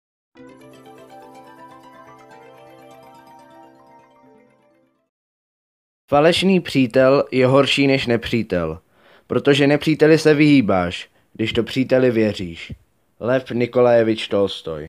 Citáty slavných 11.díl - Lev Nikolajevič Tolstoj
Dobrý den, vítejte u nového videa . Dnes uslyšíte citát od Lva Nikolajeviče Tolstoje. Užijte si video!